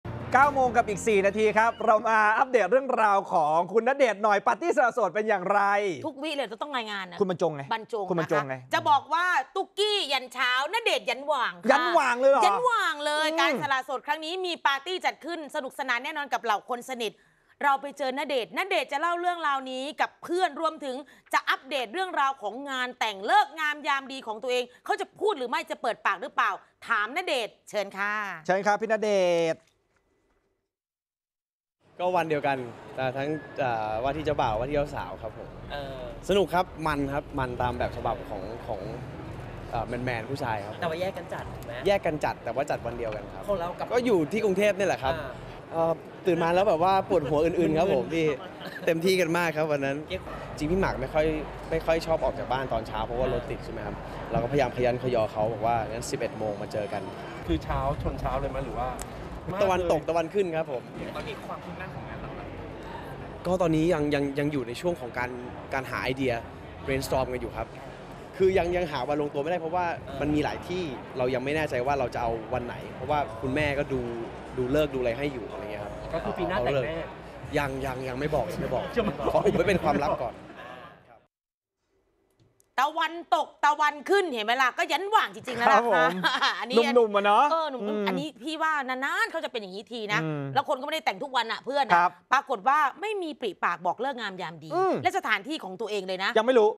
"ณเดชน์" เล่าโมเมนต์ปาร์ตี้สละโสด "หมาก" มันยันหว่าง ยังไร้ฤกษ์งานแต่งตัวเอง I 30-08-66
"ณเดชน์" เล่าโมเมนต์ปาร์ตี้สละโสด "หมาก" มันยันหว่าง ยังไร้ฤกษ์งานแต่งตัวเอง